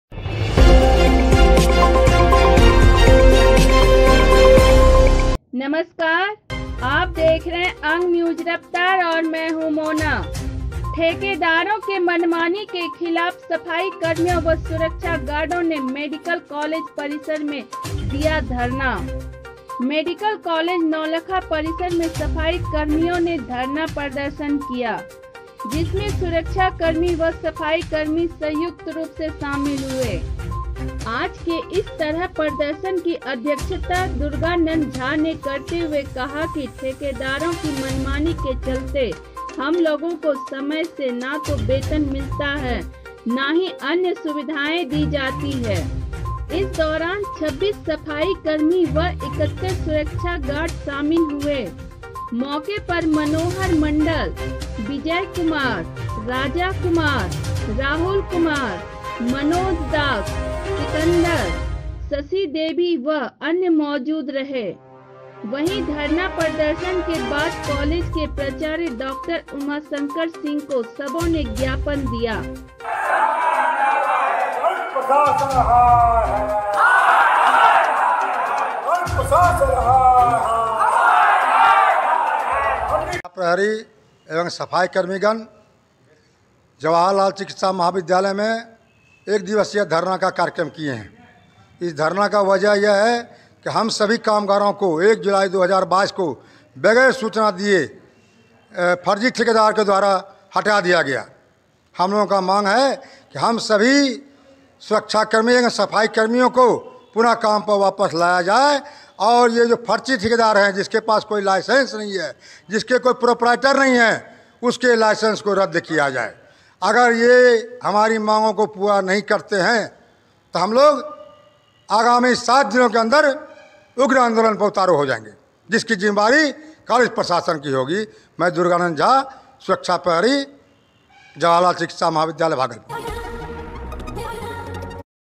ठेकेदारों के मनमानी के खिलाफ सफाई कर्मियों व सुरक्षा गार्डों ने मेडिकल कॉलेज परिसर में दिया धरना
बाईट:- सुरक्षाकर्मी दुर्गा नंदन झा